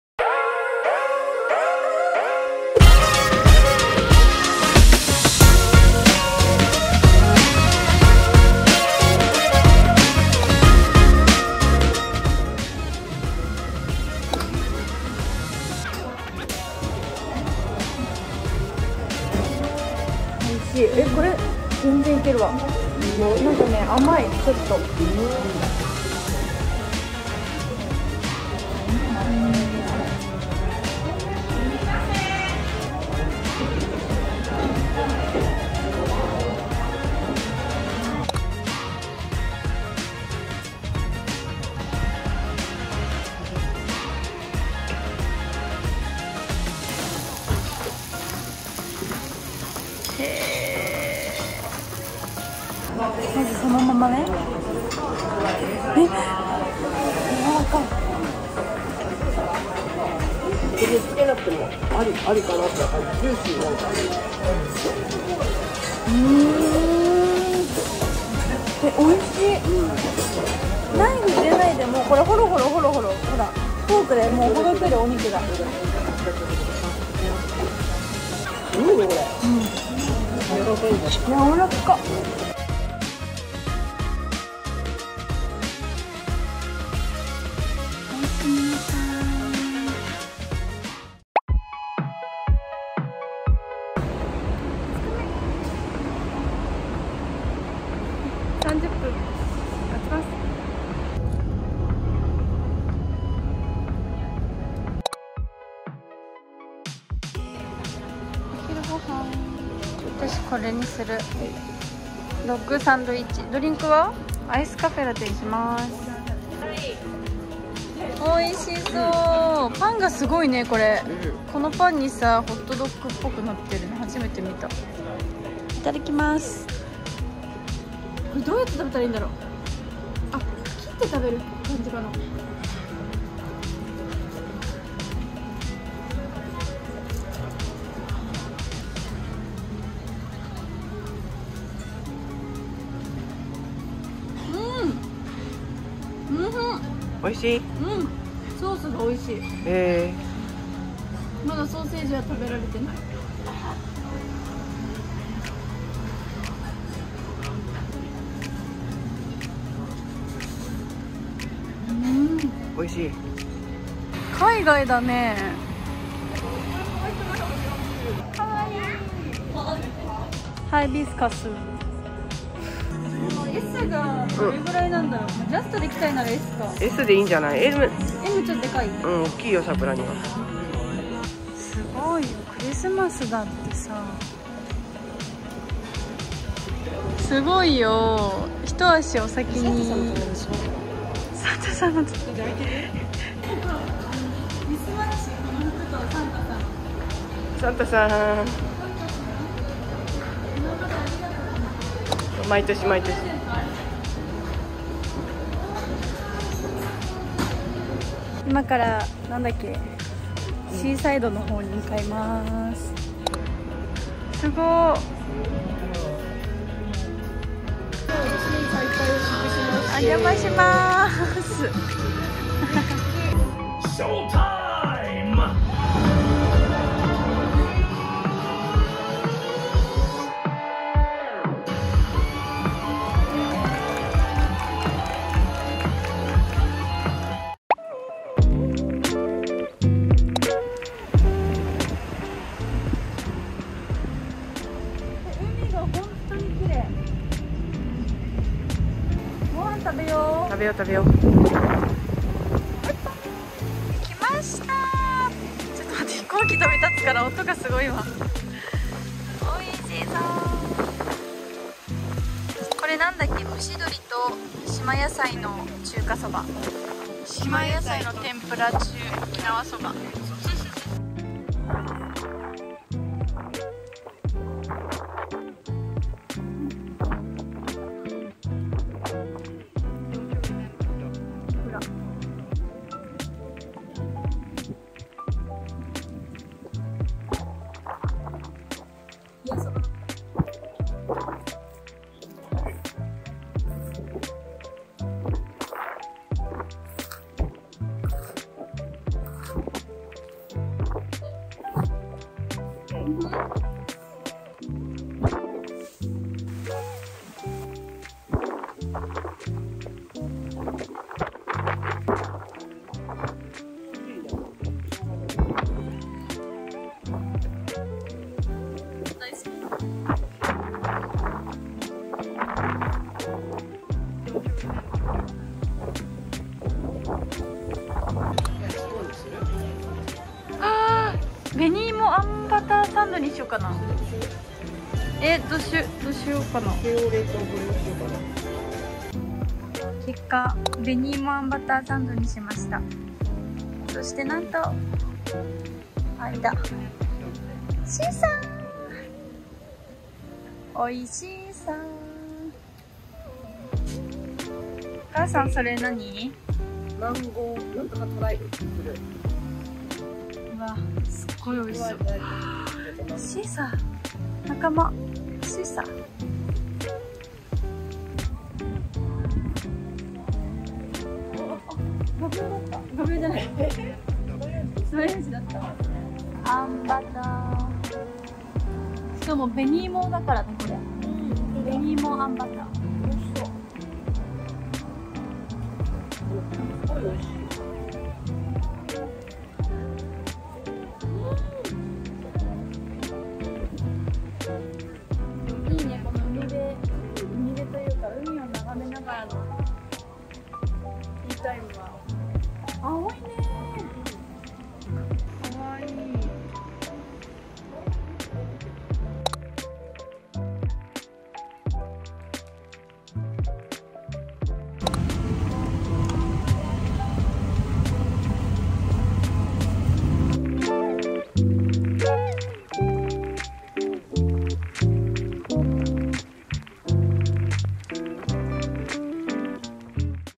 【沖縄vlog】お仕事で沖縄へ🌺お洒落なカフェや観光スポットをご紹介🌴🌞沖縄グルメ|カフェ|居酒屋|フォトスポット
沖縄vlogです🌴🌞

念願の沖縄にお仕事で行くことができたので、ついでに観光もしてきちゃいました😆🤍

たくさん食べて、お酒も呑んで、幸せだったなあ〜〜〜

海も綺麗でまた定期的に訪れたくなりました♡

母にもついてきてもらって、とっても良い思い出に☺️

たのしーさー🤫✨
0:00 DAY1
0:14 沖縄居酒屋へ
0:38 ジャッキーステーキハウスへ
1:32 DAY2
1:45 アメリカンヴィレッジの到着
2:48 フォトスポット
3:33 マジックショーが見られるレストラン
3:58 DAY3
4:06 ウミカジテラスへ
4:14 沖縄そば&中華そば
5:28 デザートを食べにカフェへ
6:48 東京に帰ります✈️